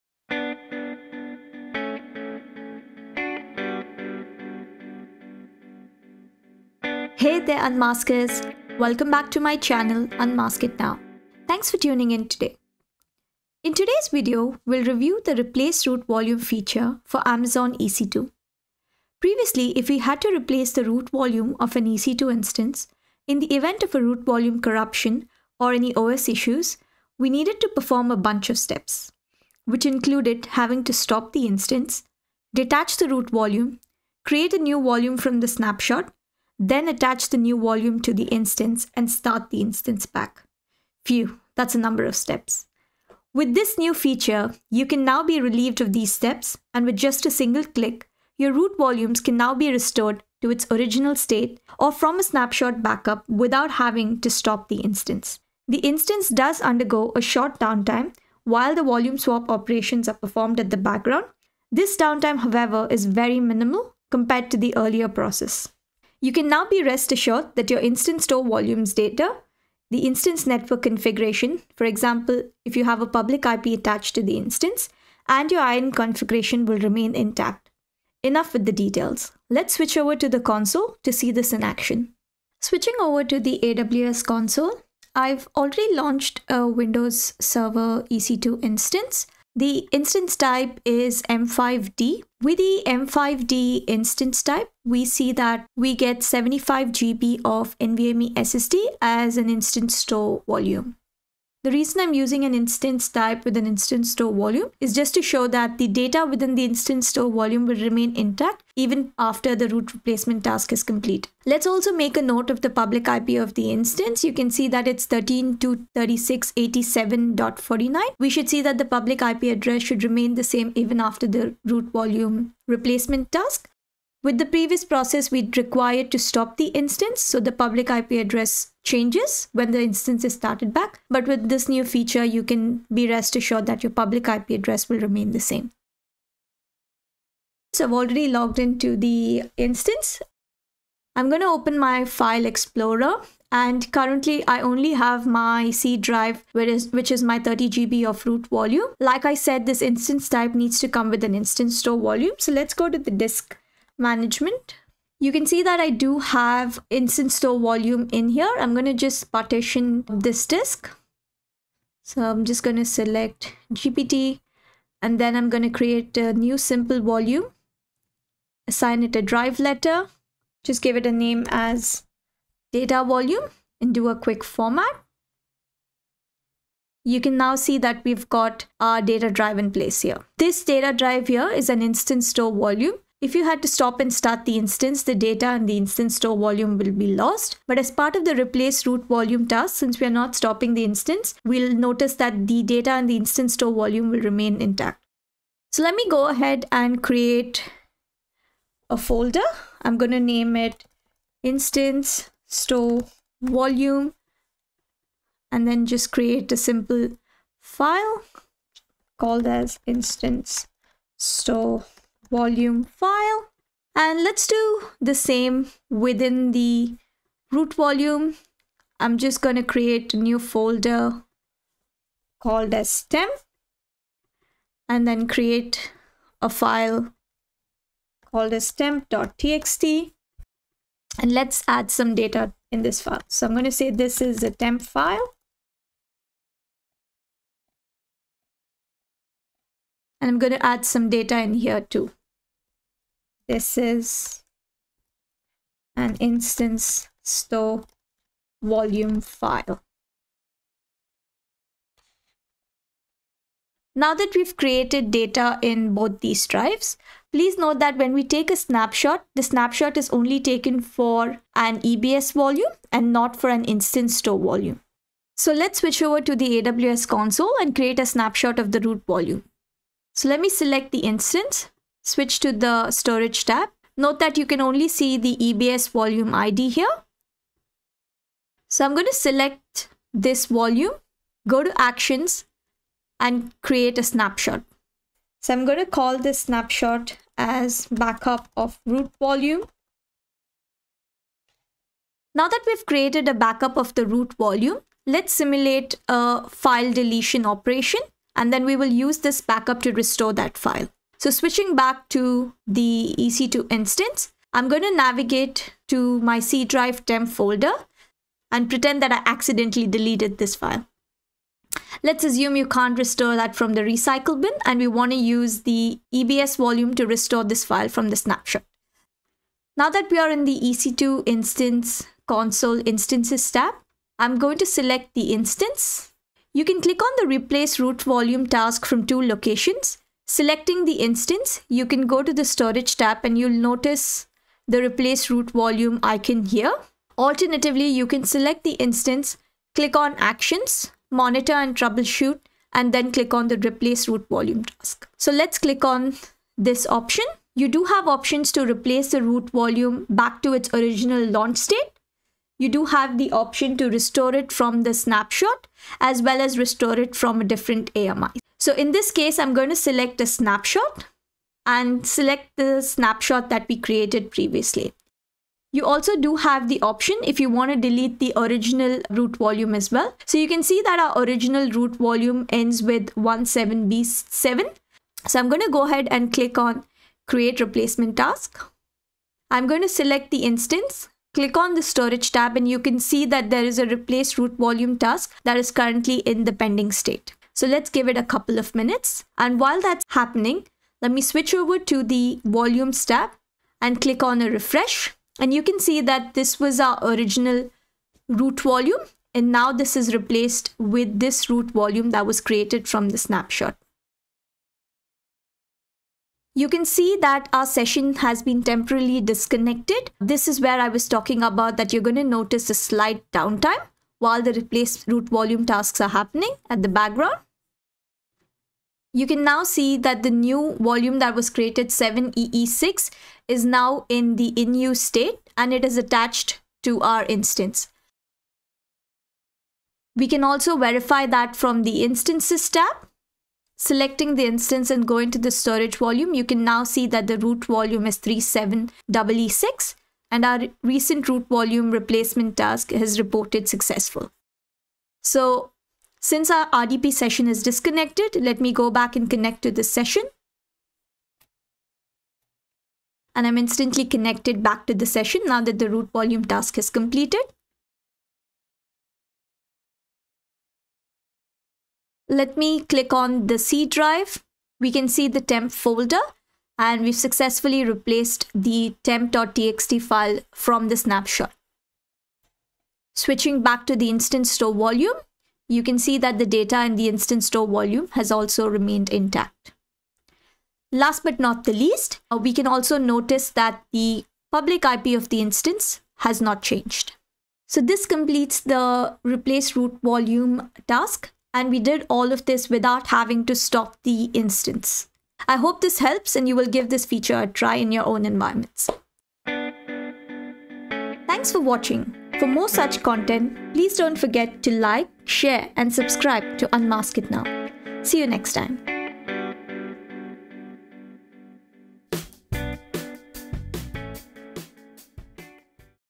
Replace Root Volume for Amazon EC2 without stopping the Instance
In this video we cover the demo of how to replace Root Volume for Amazon EC2 without stopping the Instance.